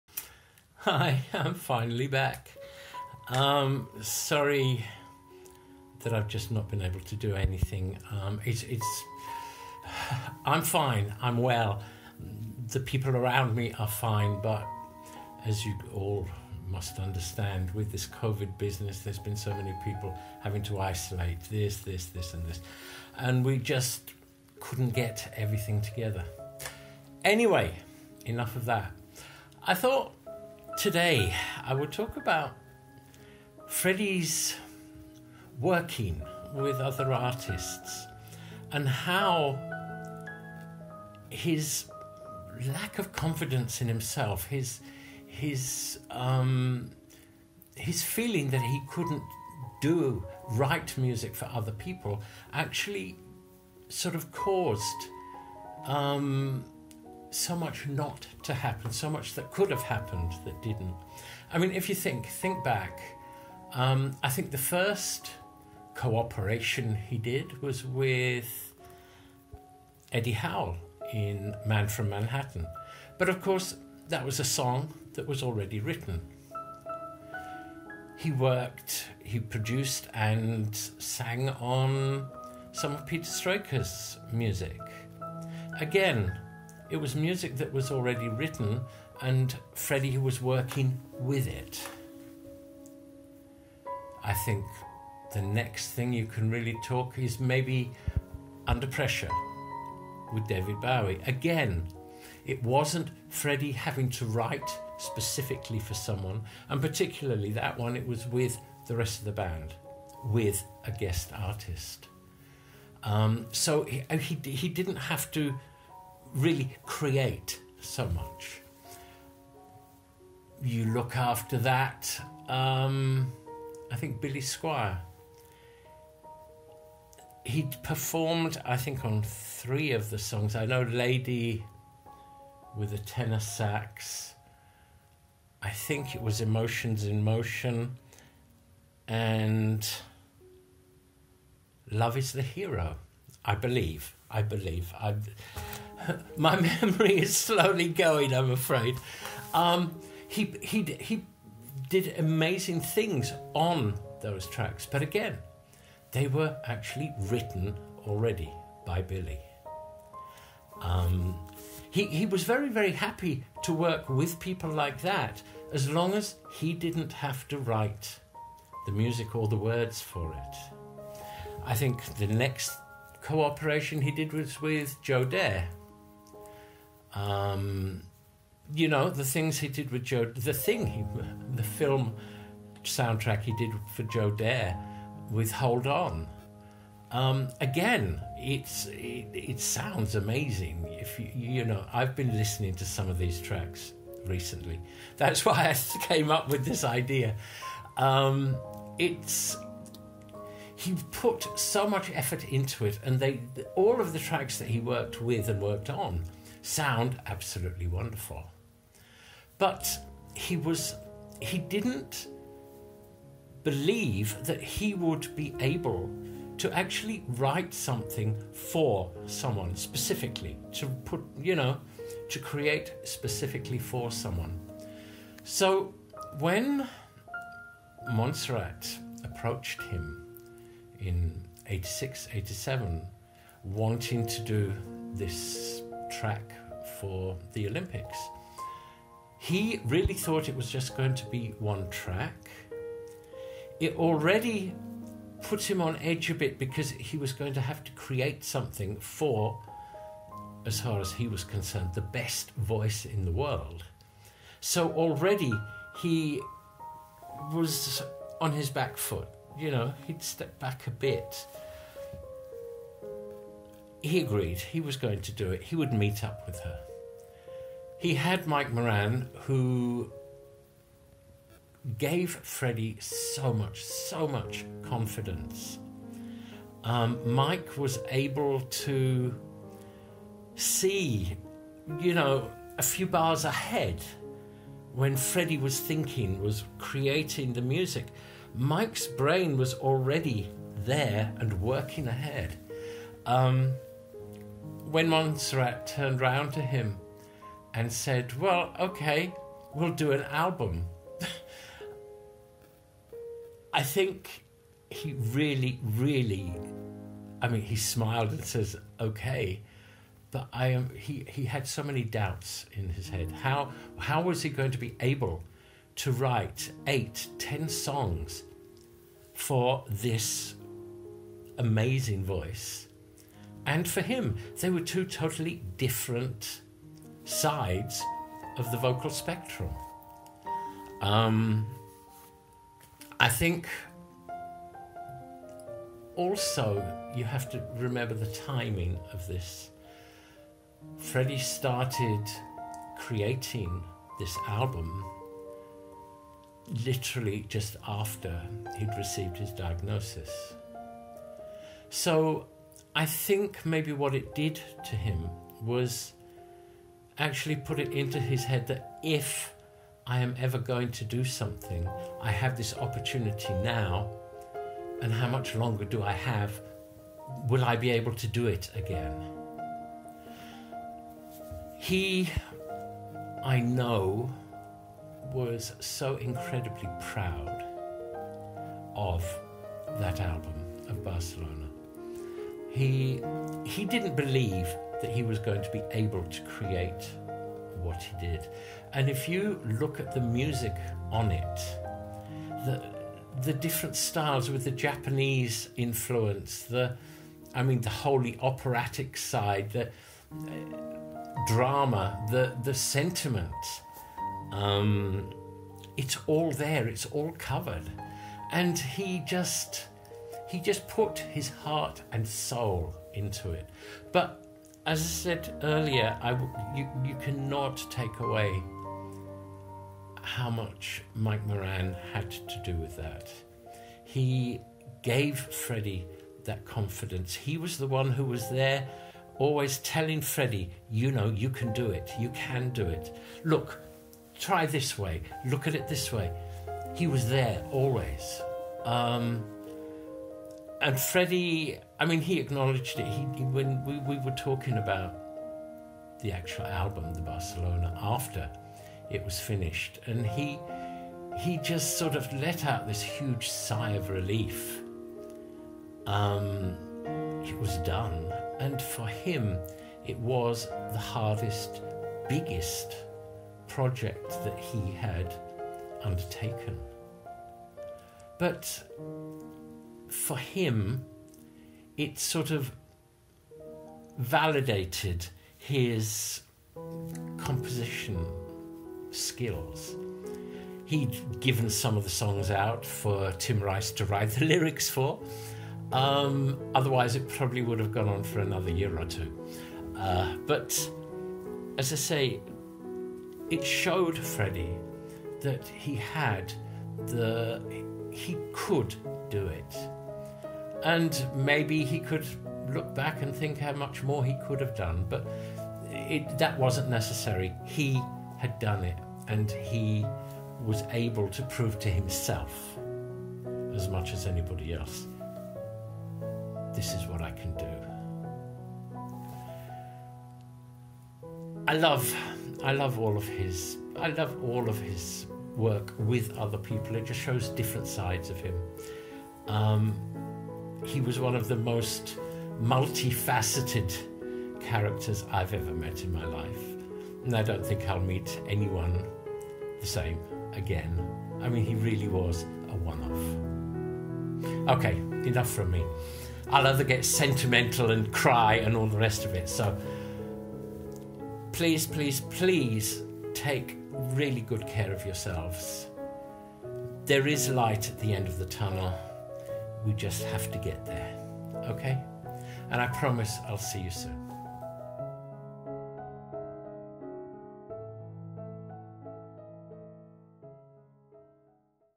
Peter Freestone - Freddie Mercury & collaborations
Sorry for not being here recently, but this pandemic is causing more problems than I dreamt of.
Today is about collaboration; why Freddie was reticent about working with other artists. I know you will all think I left out Michael Jackson, but that is only because the result of their work together never appeared in Freddie's lifetime, nor Michael's for that matter. Please take care of yourselves and I promise it won't be so long before the next one. Now getting ready to hide away for the next three weeks as per new government regulations forbidding us to venture out of our residential district!